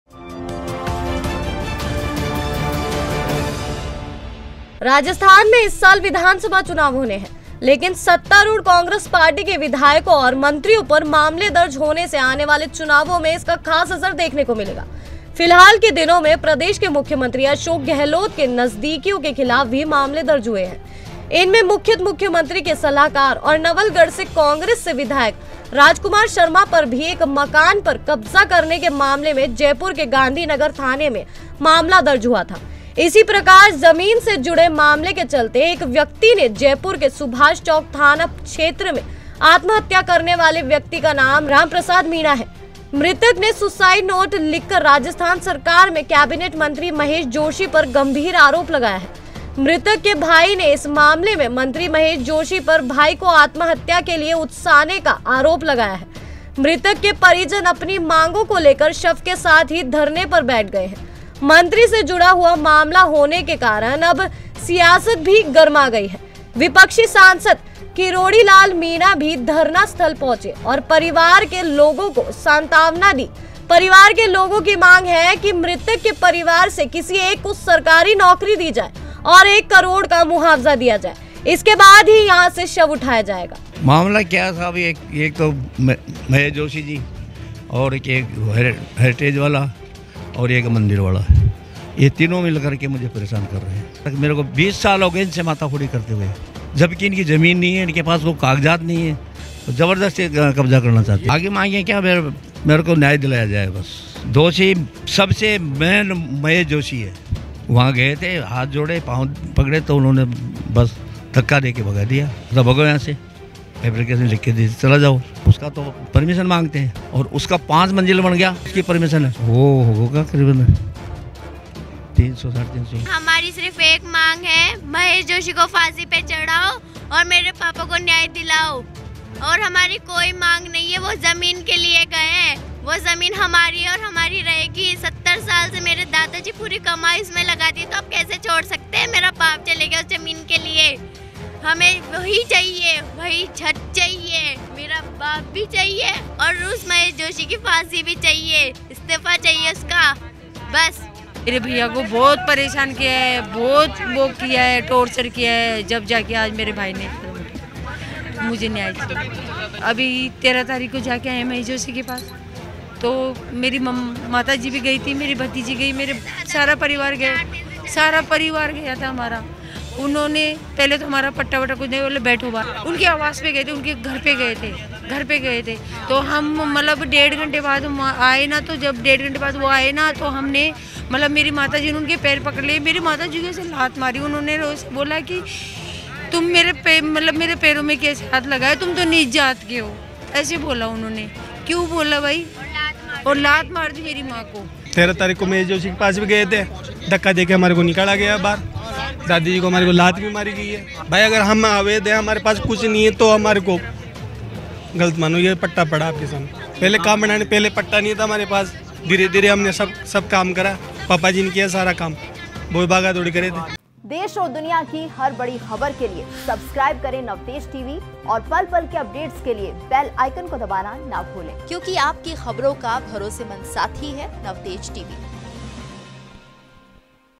Ramprasad Suicide: kirodi lal meena भी साथ, मंत्री Mahesh Joshi की गिरफ़्तारी पर अड़े परिजन Live News
Case against rajkumar sharma gehlot counseller

Familly members of ramprasad meena are adamant on action against minister mahesh joshi